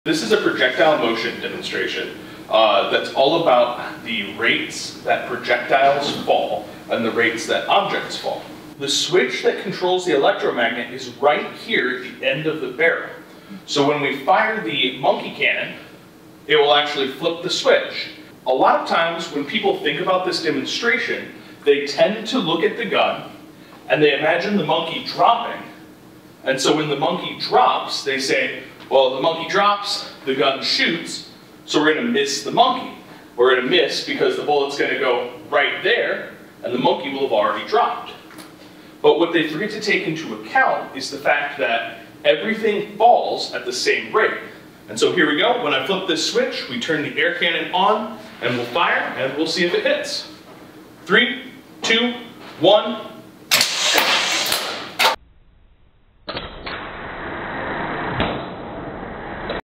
Shoot the Monkey: Projectile Motion demo
A toy monkey having a metal plate attached to his head, is suspended from an electromagnet attached to the ceiling. A projectile is fired from a tube and the monkey is released just as the projectile leaves the end of the tube. If the tube is aimed directly at the monkey initially, the two will collide in mid-air.